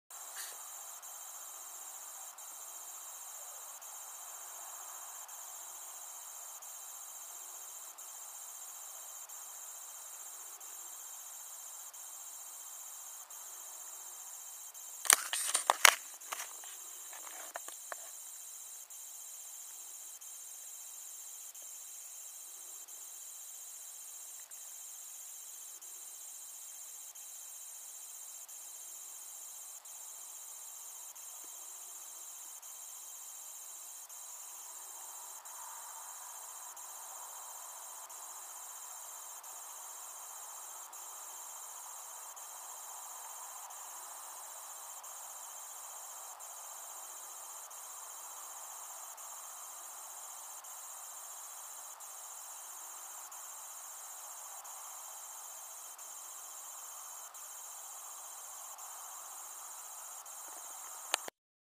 atkinson family christmas lights 2010 test
A test run for the 2010 display about half of the lights done so far!!